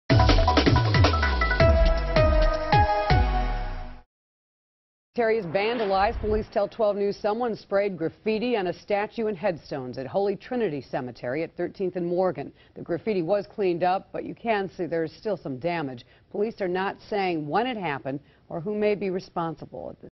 Milwaukee cemetery vandalized
Graffiti was reported at Holy Trinity cemetery.